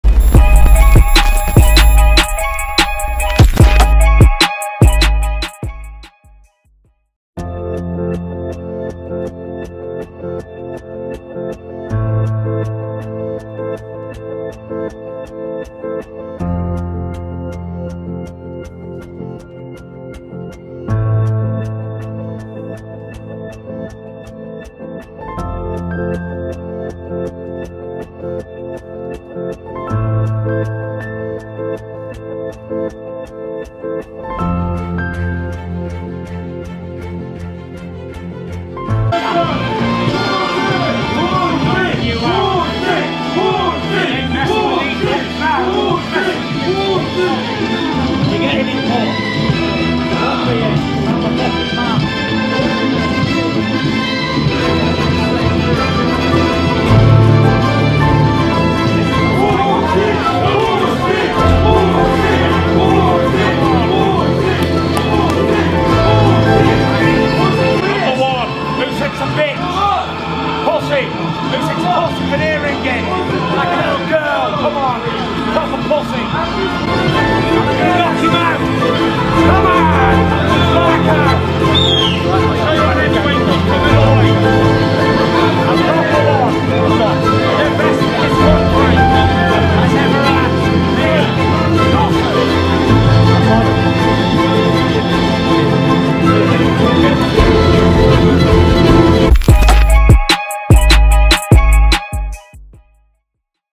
"YOU'RE A B****!" TYSON FURY AND OLEKSANDR USYK CLASH HEADS IN HEATED FACE OFF
Tyson Fury and Oleksandr Usyk came face to face at the press conference in London today, and safe to say things got a little heated.